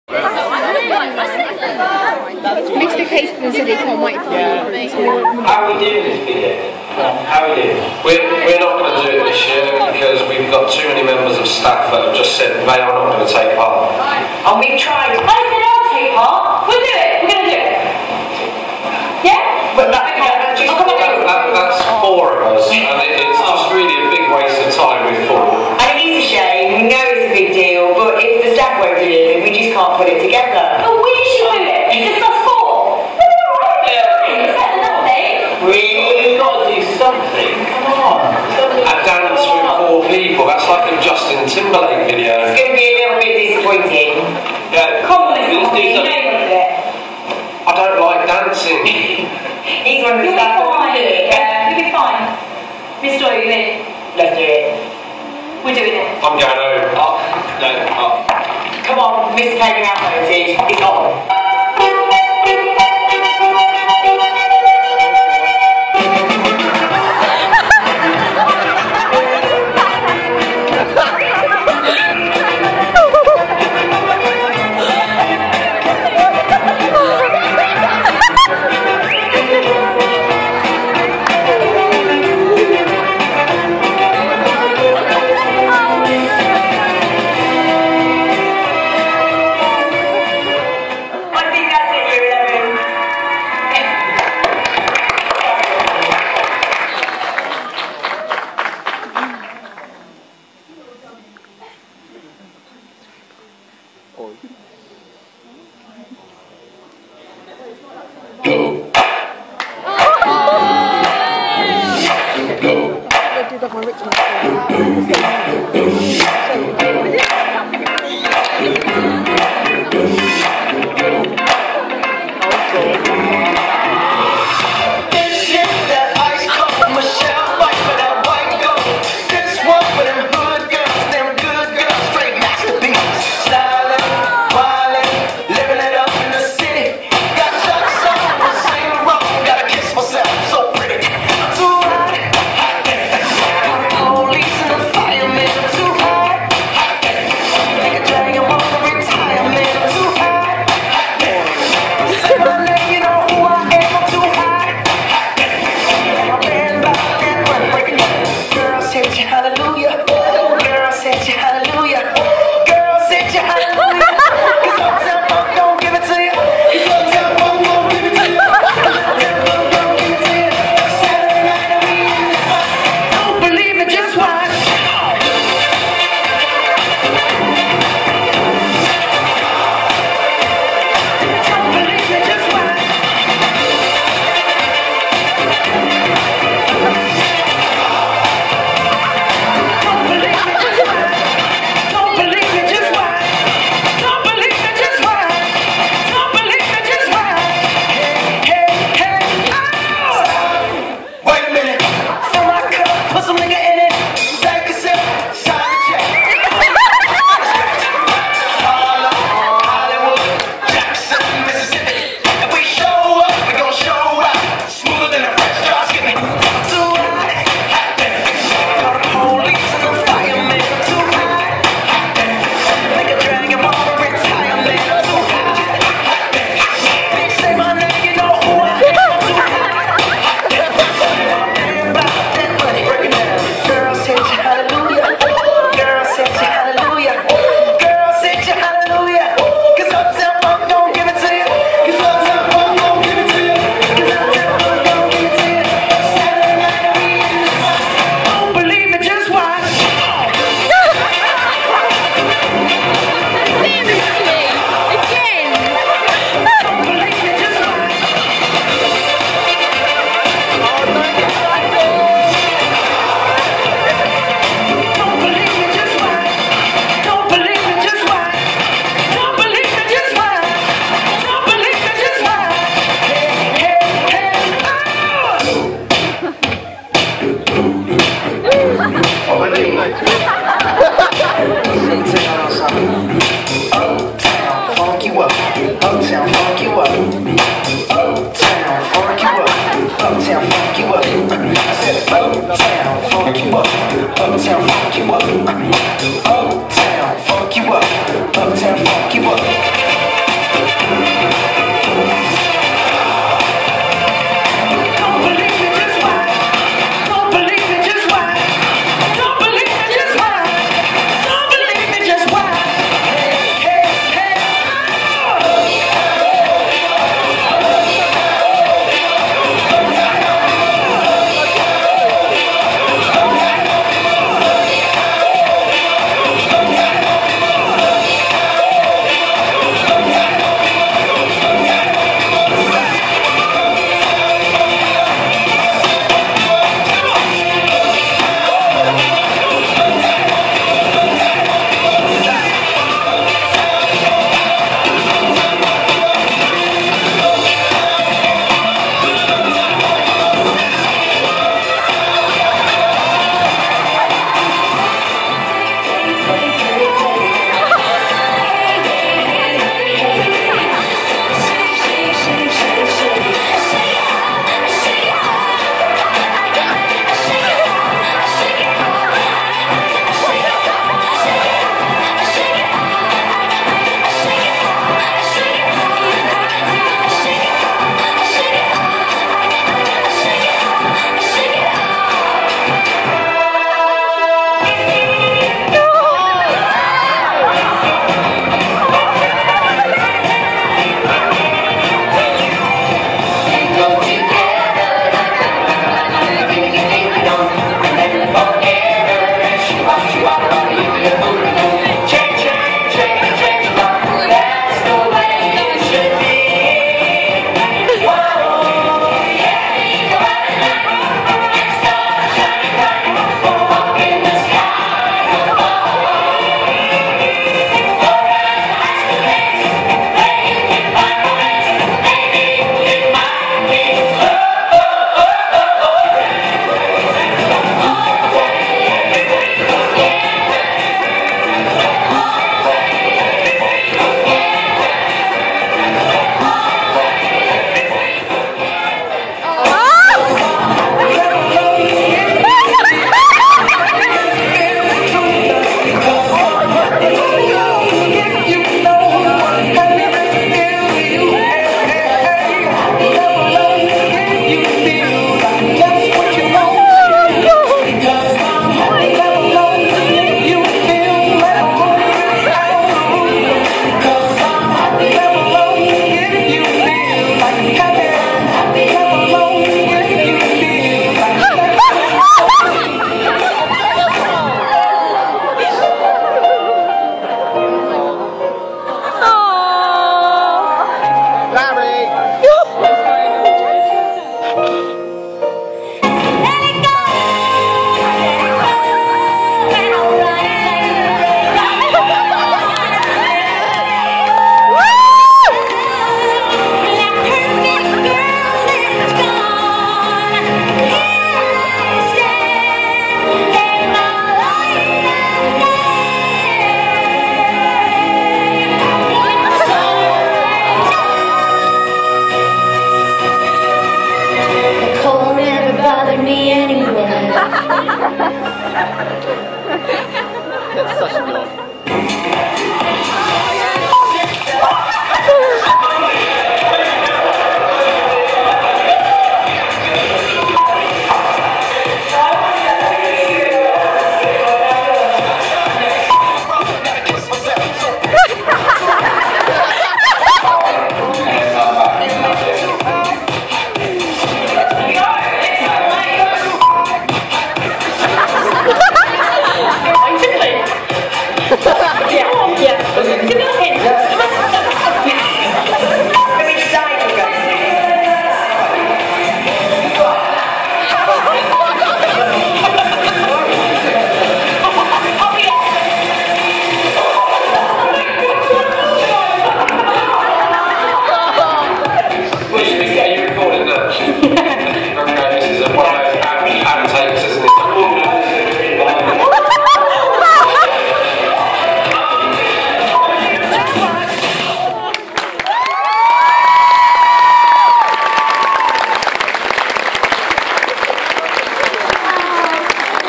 The Causeway School Class Of 2015 Leavers Video
This is the leavers video from staff to the class of 2015.  Not very good quality as was taken via phone.